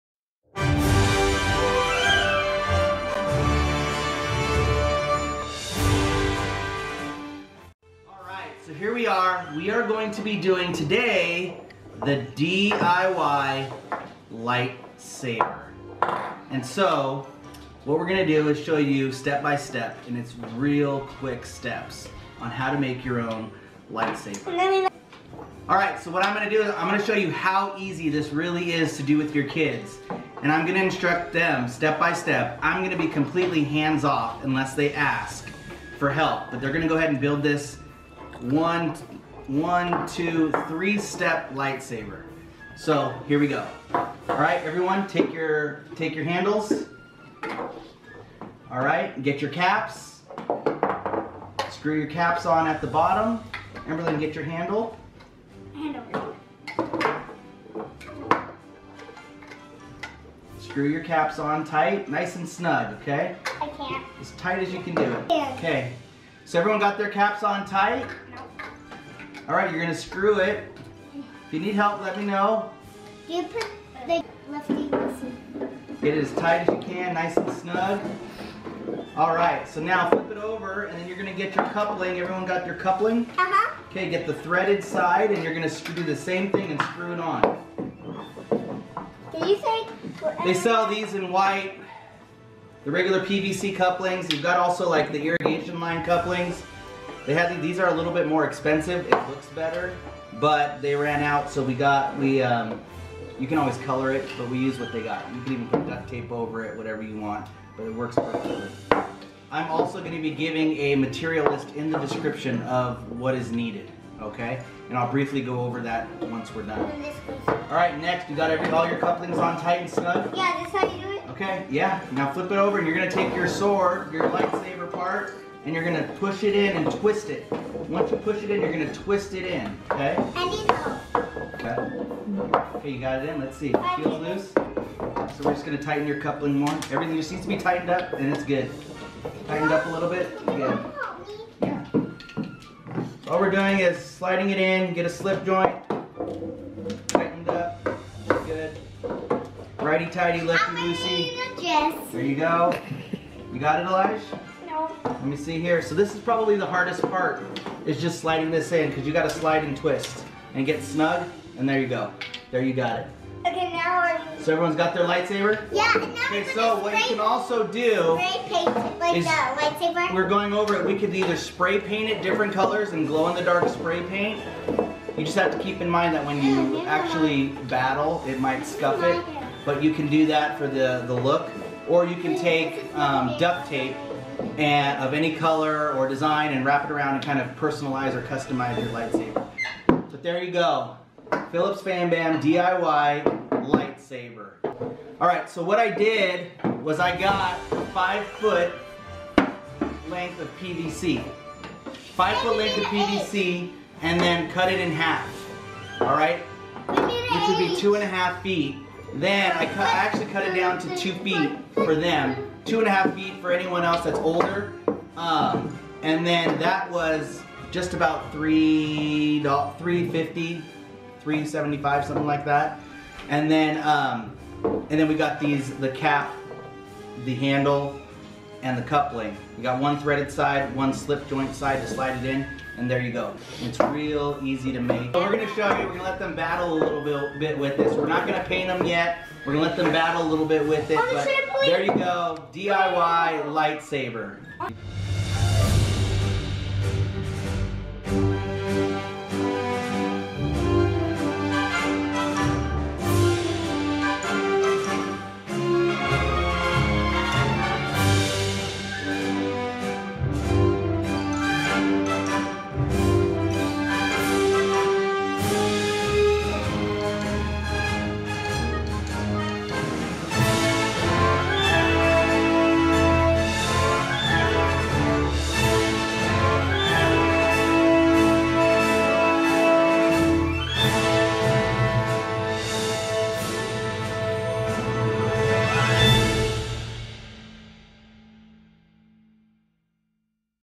HOW TO MAKE A PVC LIGHTSABER | DIY | PHILLIPS FamBam HowTo
Here is a quick and easy way to make a light saber with your kids. I give you 3-4 easy steps to making a light saber from scratch.

1- 1" x 12" PVC Threaded PIPE or IRRIGATION PIPE
1- 1" Female threaded inlet / Slip Joint
1- 1" PVC CAP or IRRIGATION PIPE CAP
1- 1" x 2' PVC PIPE

OPTIONAL:
1- 1" PVC CAP for top of Light Saber
1-  1" Diameter LED light to stick in the handle and use for lighting up the PVC